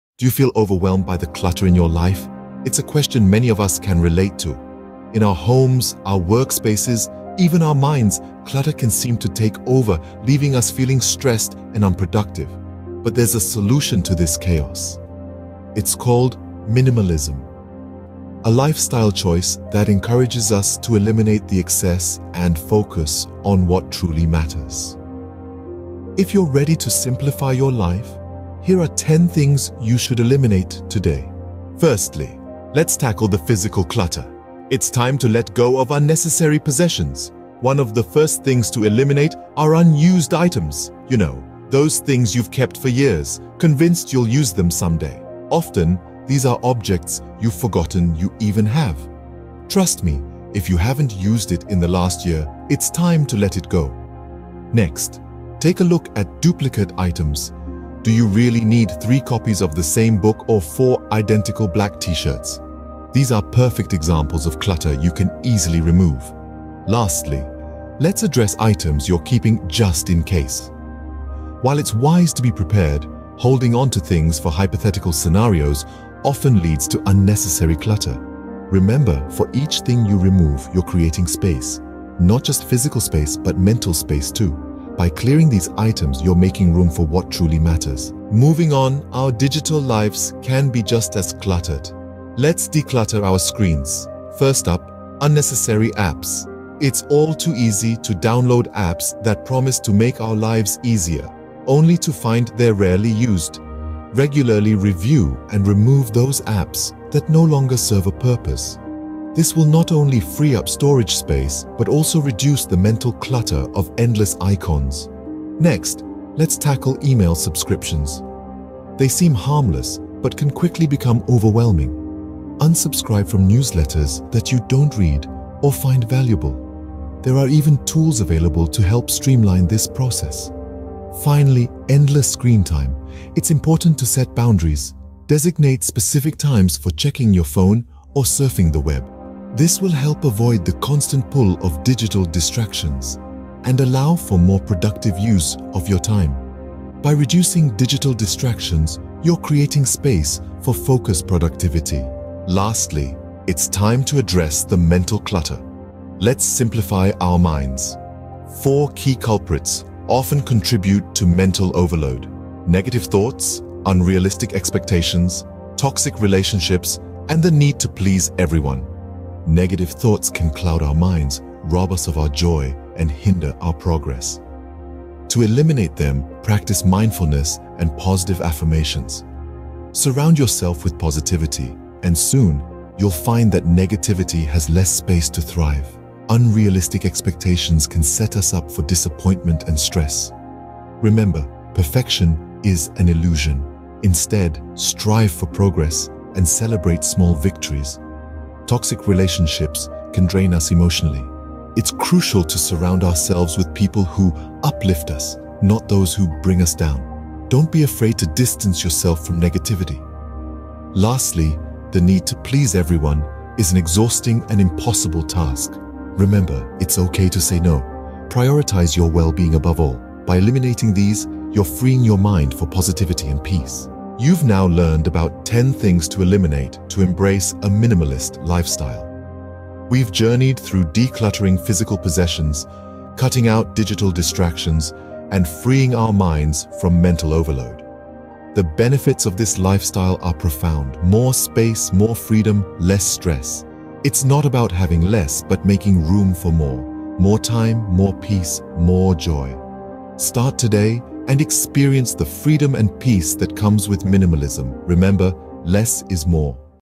Embrace Minimalism: 10 Things to Eliminate
Embrace minimalism and simplify your life by eliminating physical clutter, digital distractions, and mental overload. Learn about the 10 things you should eliminate today to embrace a minimalist lifestyle and experience more space, freedom, and peace.